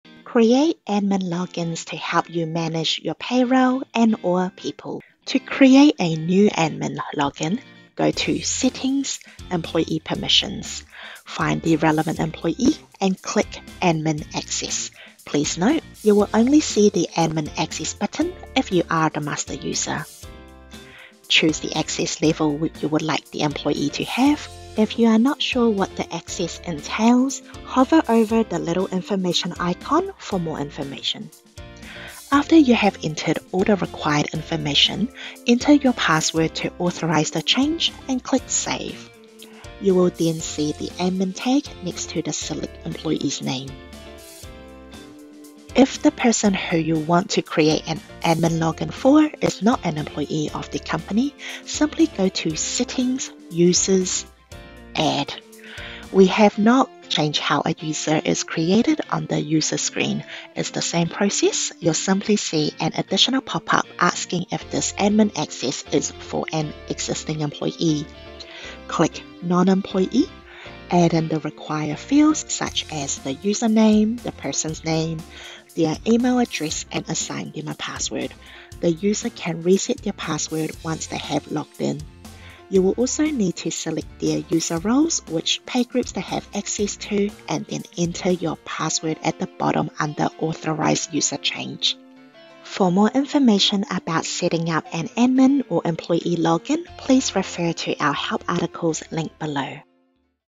Create admin logins
Create admin logins to help you manage your payroll and/or people.